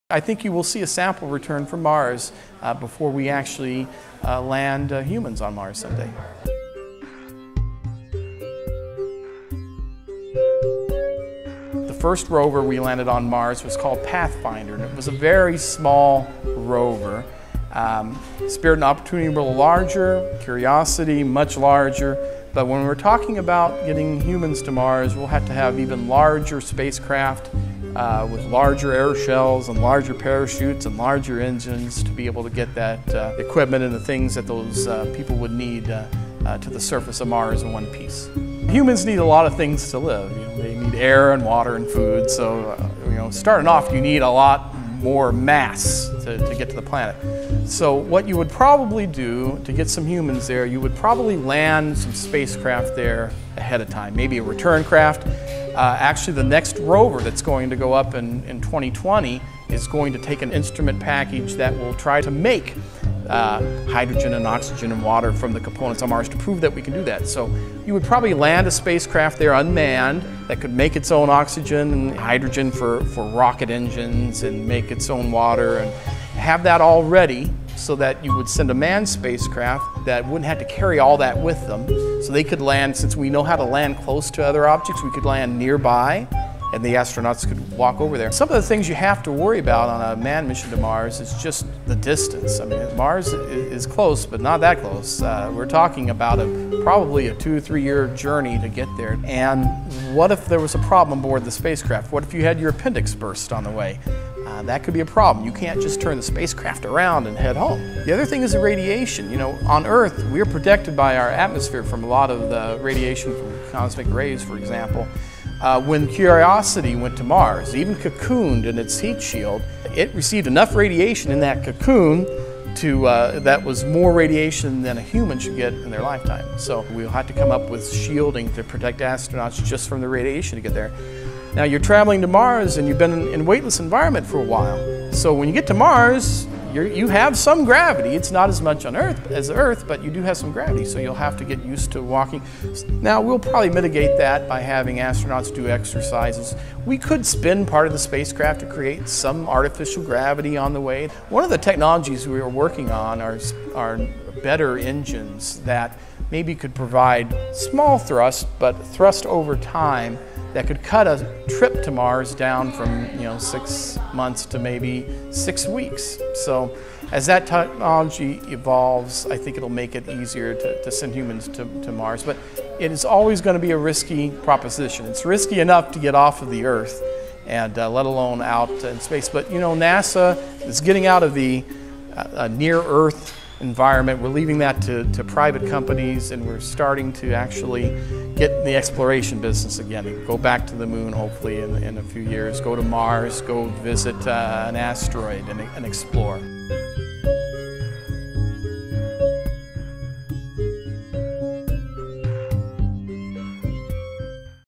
Rovin' Around Mars: Mission to Mars
Join us with NASA Ambassador Joel Knapper he talks about "The Martian," and the Mars rover program.  In this video, Joel discusses the possibility of sending humans to Mars, and the challenges that come with that.